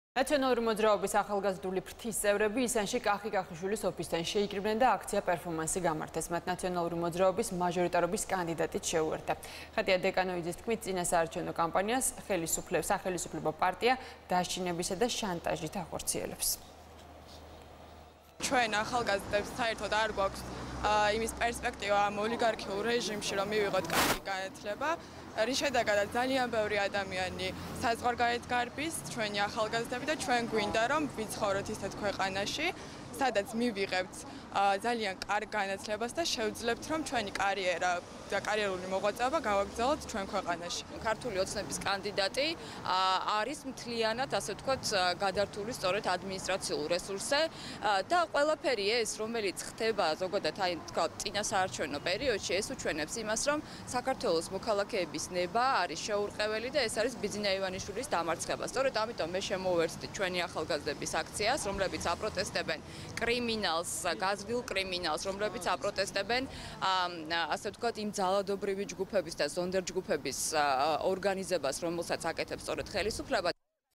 არჩევნები2020 „ნაციონალური მოძრაობის“ აქცია
„ნაციონალური მოძრაობის“ ახალგაზრდული ფრთის წევრები ისანში კახი კახიშვილის ოფისთან შეიკრიბნენ და აქცია-პერფორმანსი გამართეს.

მათ „ნაციონალური მოძრაობის“ მაჟორიტარობის კანდიდატიც შეუერთდა. ხატია დეკანოიძის თქმით, წინასაარჩევნო კამპანიას სახელისუფლებო პარტია დაშინებითა და შანტაჟით ახორციელებს.